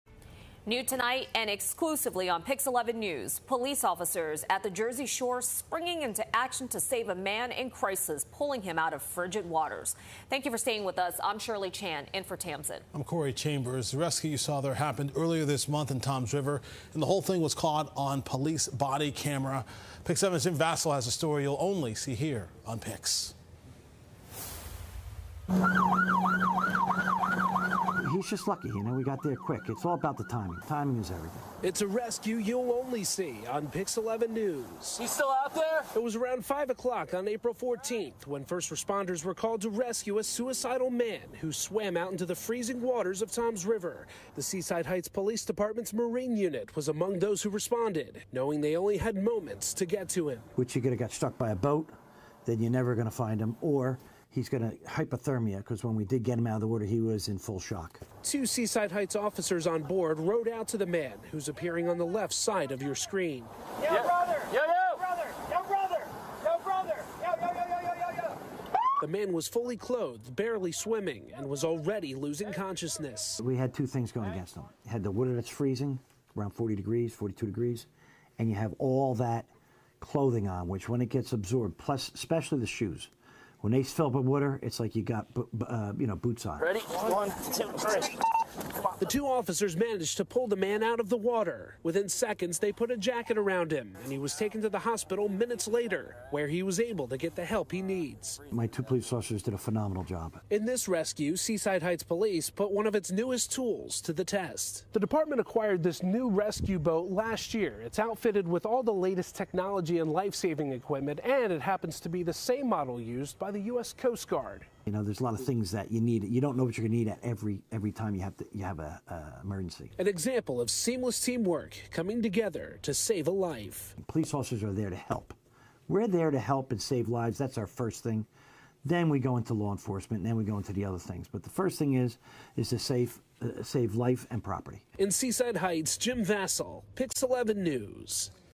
Seaside Heights police rescue man in crisis from freezing ocean
First responders are being hailed as heroes after saving a man in crisis, pulling him out of freezing cold waters.